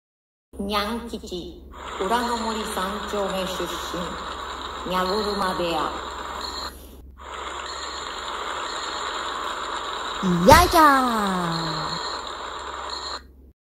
「にゃん吉関」裏の森、大相撲大会。出場者の、説明。アニメーションby Ken nogawa
出身→裏の森３丁目

部屋→にゃ車部屋

化粧回しに、渋い黒に、吉の、もじと、富士山(でかく出る).